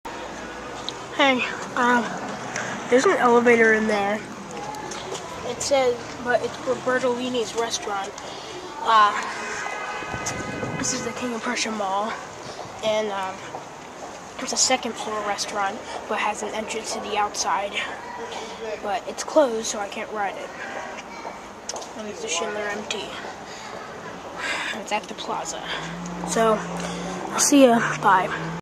A Perfectly Good Elevator Ride That Can't Happen
This is a Schindler MT elevator that is to bring people up to Bertolini's, a resteraunt that used to be on the second floor in The Plaza at KOP that had an entrance on the first floor, but we cant ride it because Bertolini's is closed and nobody wants to have that space.